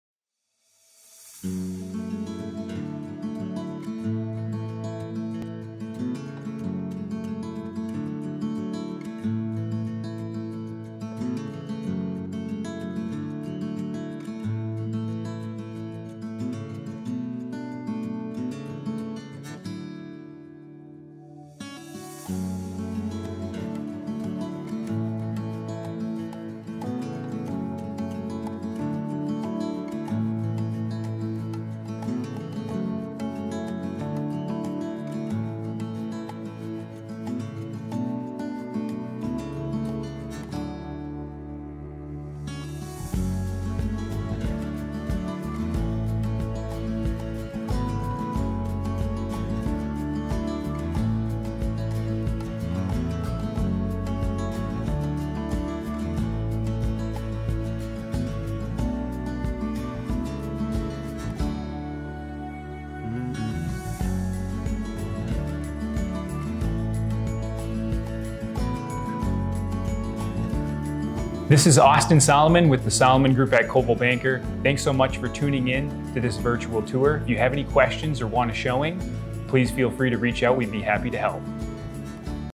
1815 Fleming St | Wausau, WI
Spacious 3-bedroom ranch home just listed on Wausau's Southeast side!

Listed by Coldwell Banker Action.